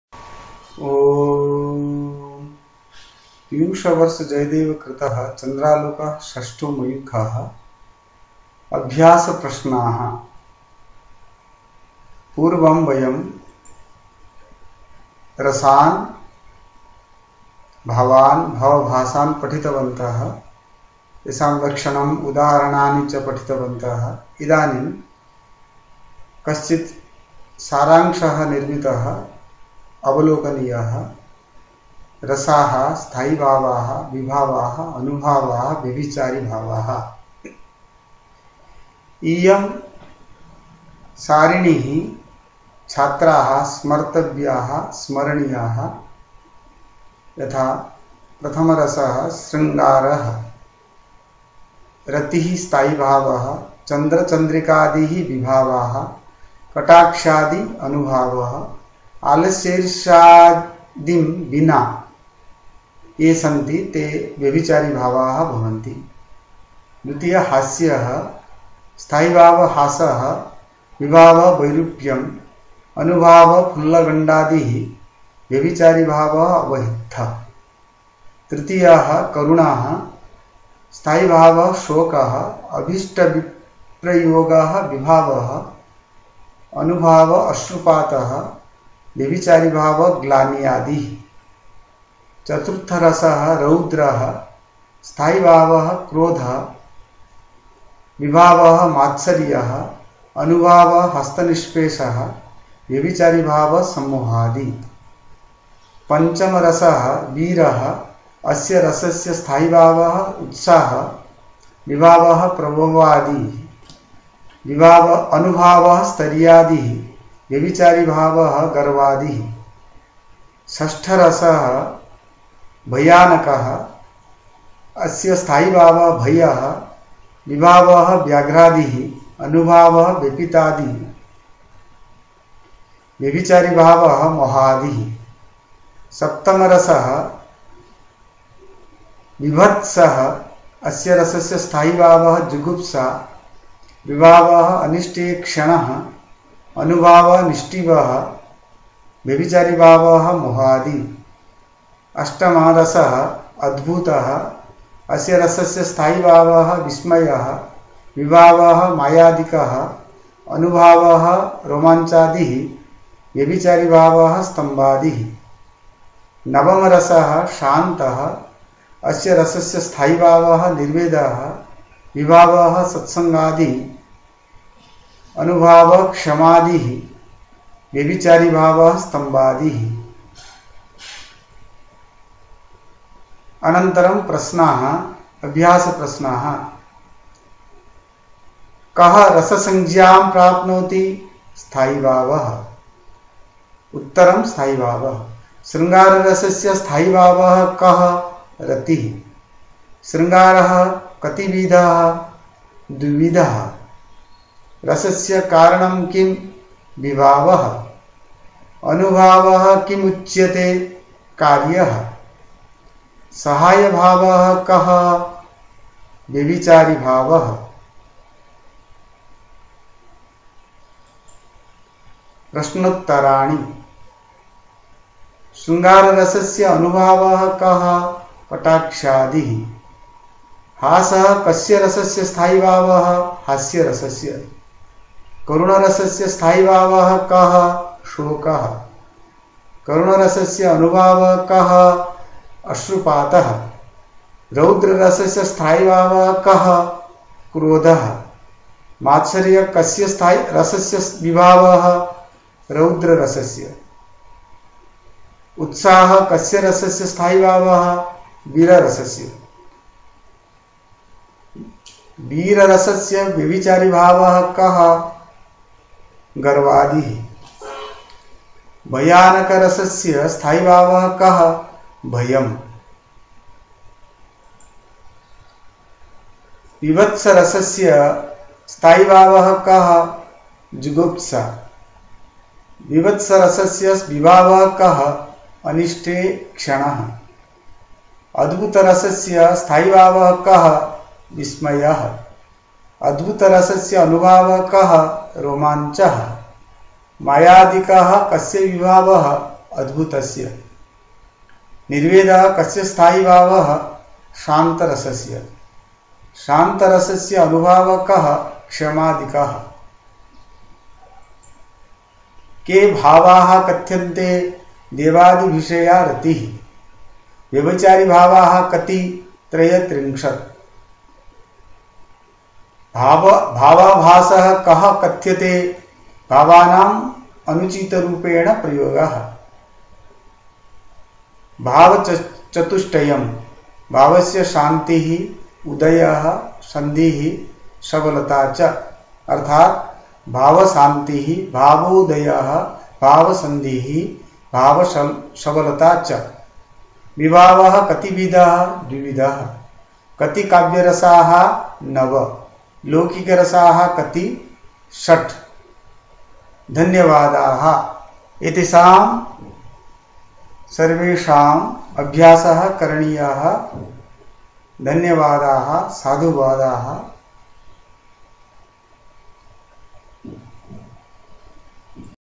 sanskrit Class 12 साहित्य शास्त्र रसाभास भावाभास प्रश्ना: (Rasabhas Bhavabhas Prashna)
संस्कृत शिक्षा विभाग राजस्थान सरकार कक्षा 12 साहित्य शास्त्र रसाभास भावाभास प्रश्ना: का ऑनलाइन अध्ययन